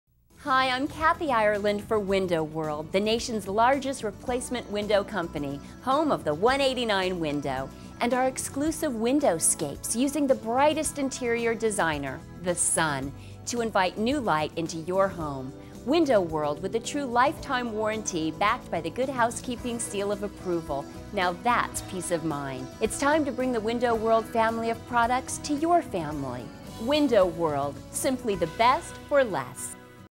Kathy Ireland for Window World Ocala
Kathy Ireland explains why WIndow World Ocala offers the best window replacement service in all of Florida